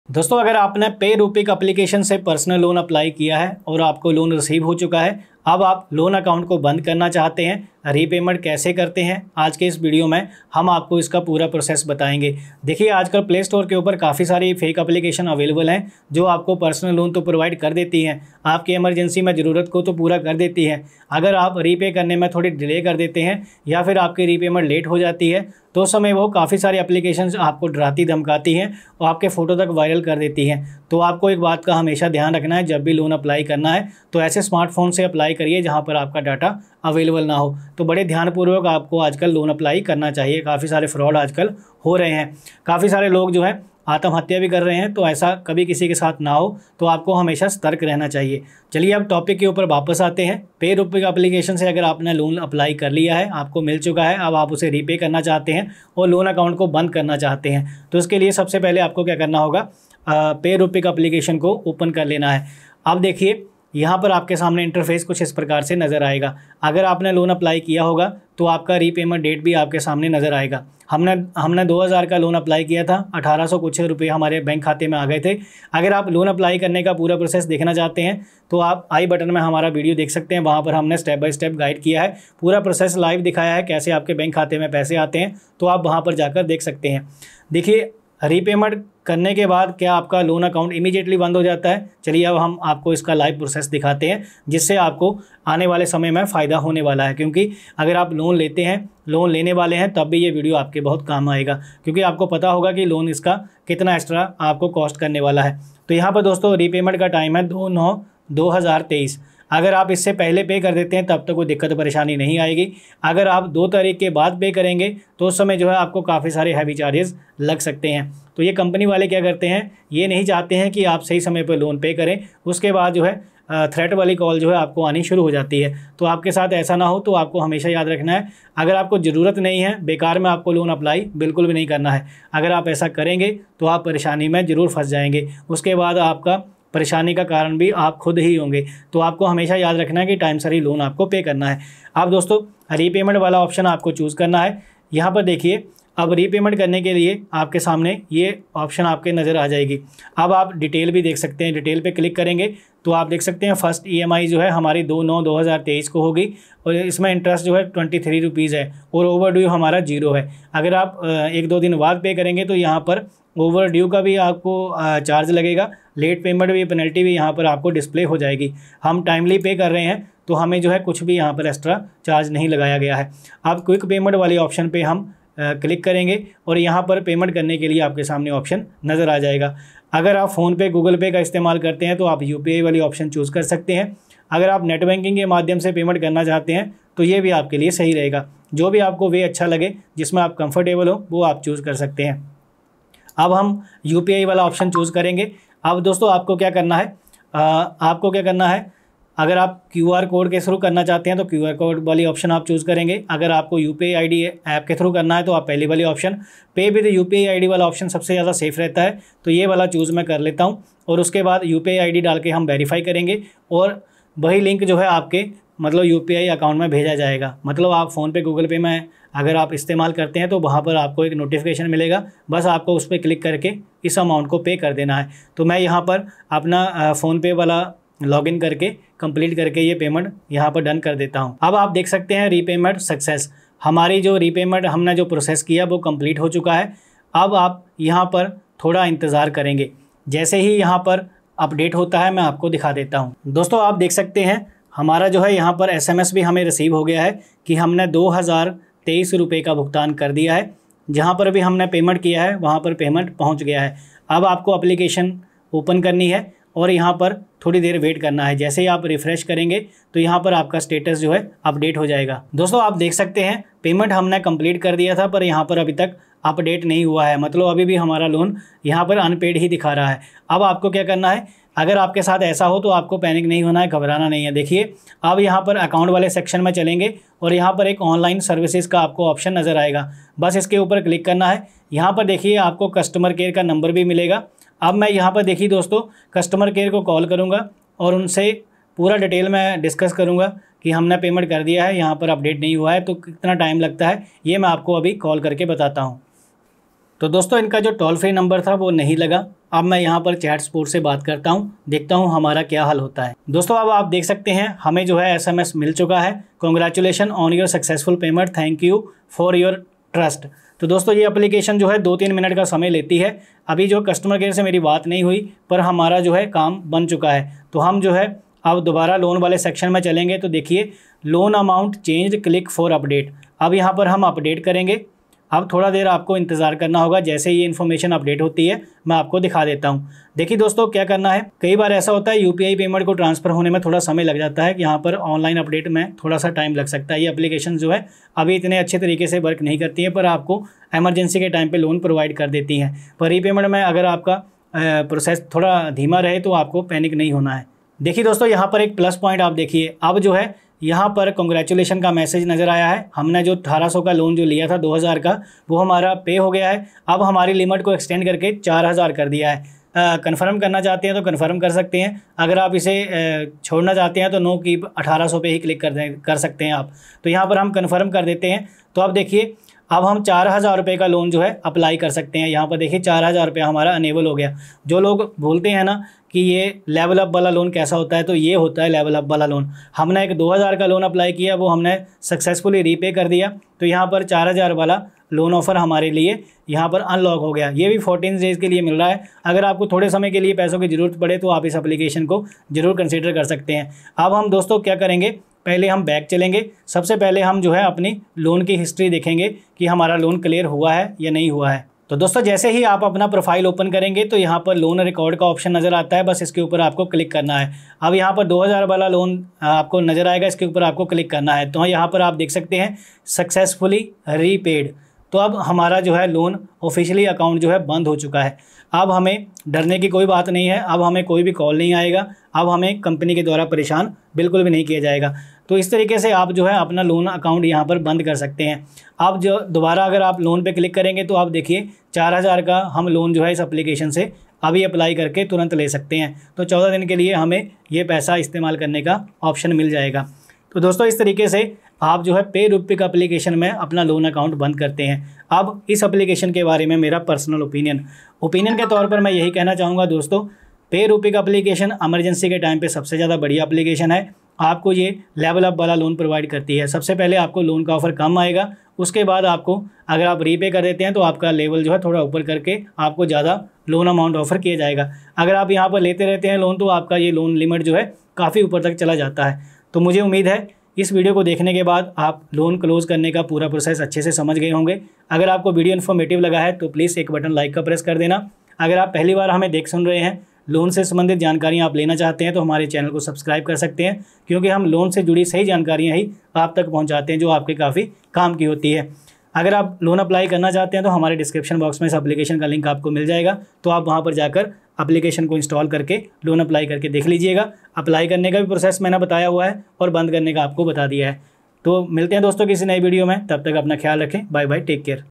PayRupik Loan Kaise Band Kare | PayRupik Loan App honest Review 2023 | PayRupik Loan le ya Nahi ?
PayRupik Loan Kaise Band Kare | PayRupik Loan App Review 2023 | PayRupik Loan le ya Nahi ?

Loan App Link:-
Other Best Loan option Without Income Proof

Payrupik Loan App – your ultimate financial companion! In this video, we dive deep into the features and benefits of this innovative loan application. Payrupik offers seamless access to quick, hassle-free loans tailored to your needs. Discover how to apply effortlessly, track your loan status, and access competitive interest rates. With its user-friendly interface and robust security measures, Payrupik ensures your financial data is safeguarded. Say goodbye to lengthy paperwork and endless queues; this app revolutionizes the lending process. Learn how Payrupik empowers you to achieve your financial goals with ease. Watch our video and embark on a journey towards financial freedom today!